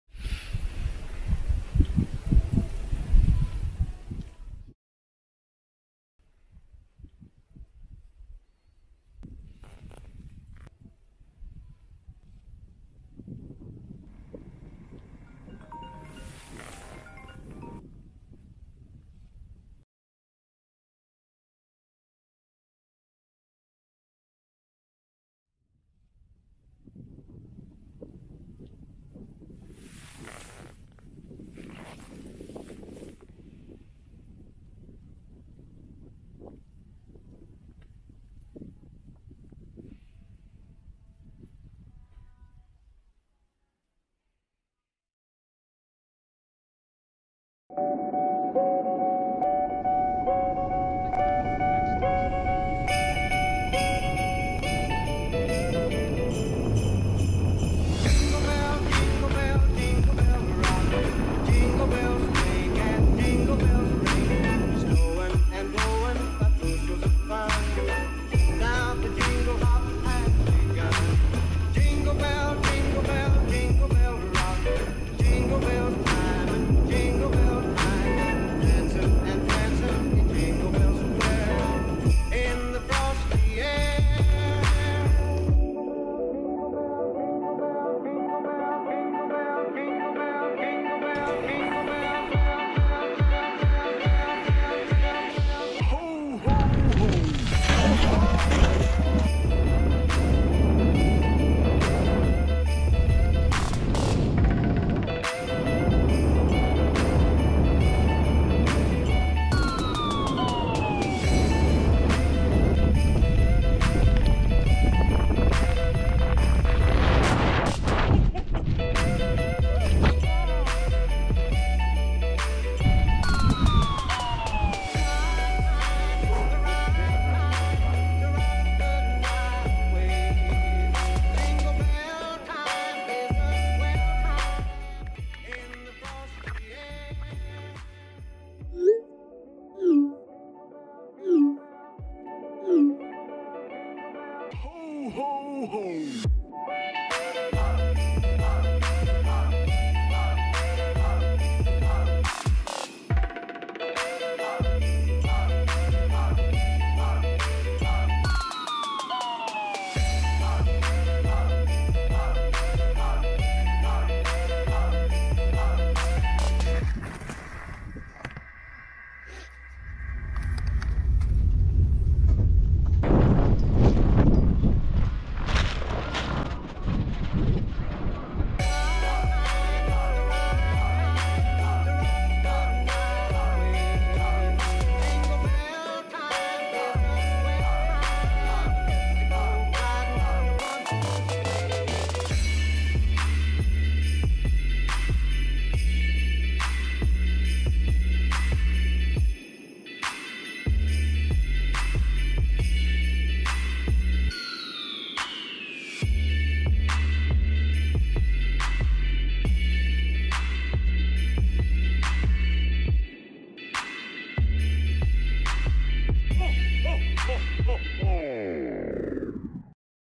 🎅 Santa Claus is Coming on 🏂 snowboard with "Japow" ( 4K UHD) | Short Film (2021)
Santa has a duty every year but this time he chose snowboard rather than reindeer.
Reindeer can stay with his family.

Merry Christmas !

Filmed by GoPro HERO 10, Osmo Pocket
Edited by Wondershare Filmora
Filmed in Kagura Snow Resort (Nigata, Japan)